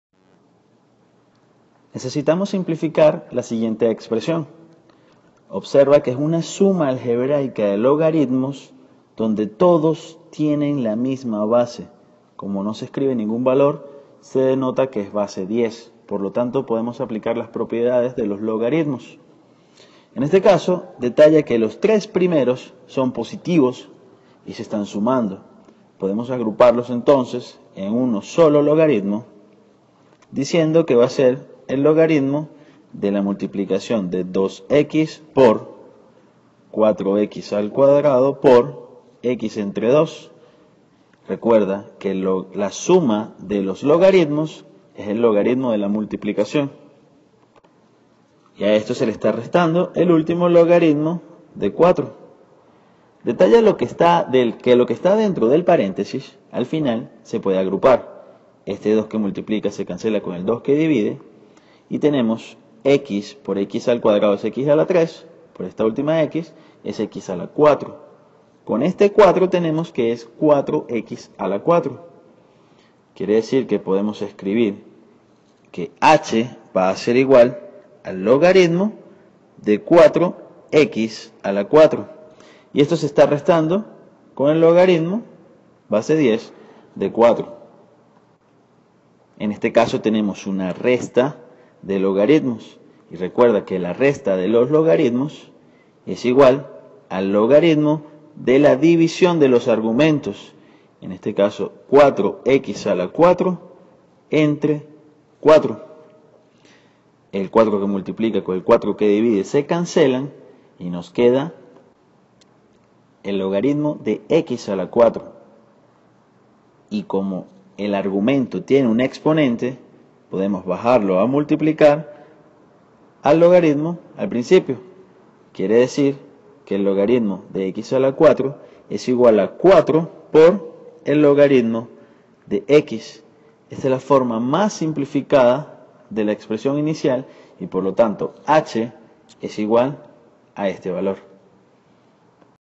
Expresiones Logarítmicas
Acá verás como usar las propiedades de los logaritmos para simplificar expresiones logarítmicas.